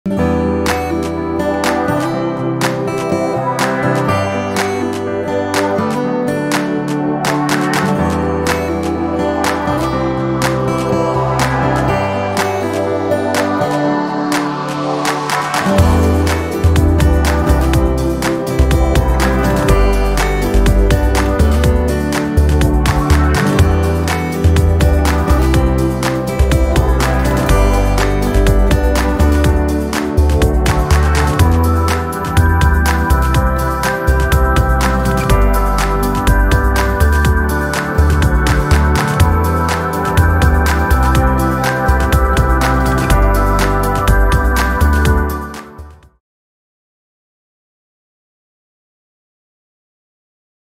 Lug Butterfly Valve Introduction
Experience the industrial strength of our Heavy-Duty Lug Butterfly Valve, built for rigorous applications. This valve features a robust construction and a precise sealing mechanism, ensuring leak-proof performance in water distribution, oil and gas, and industrial processing. See the valve in action and discover its key benefits and features.